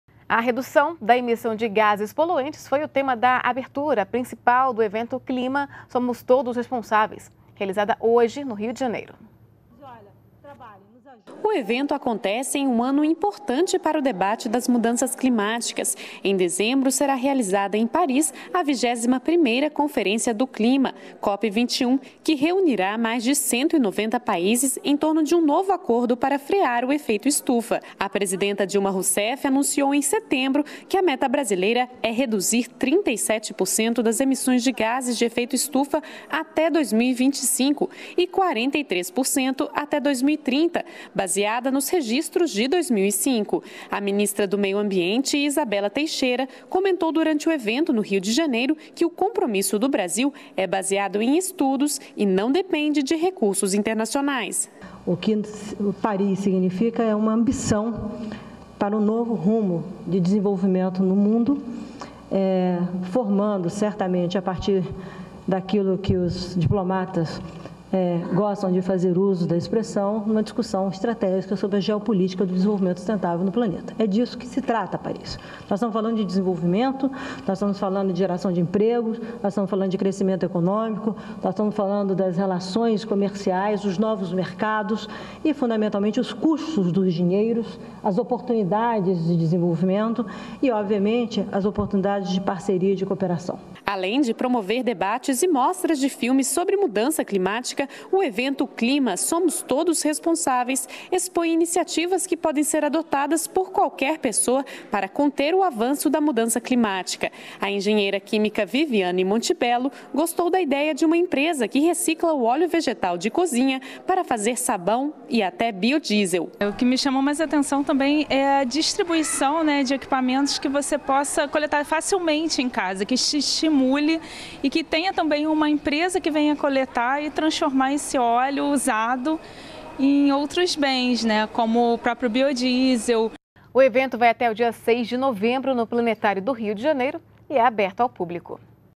Rio de Janeiro sedia evento sobre mudanças climáticas até 6 de novembro
NBR NOTÍCIAS - 03.11.15: Acontece no Rio de Janeiro, até o dia 6 de novembro, a semana do clima "Mudanças Climáticas: Somos Todos Responsáveis". O tema da abertura foi a redução dos gases poluentes. O evento tem uma programação voltada para as mudanças climáticas e as ações que tem sido feito para amenizar seus efeitos. O ato acontece em ano importante, pois antecede a 21ª Conferência do Clima das Nações Unidas (COP 21), que ocorre de 30 de novembro a 11 de dezembro, em Paris.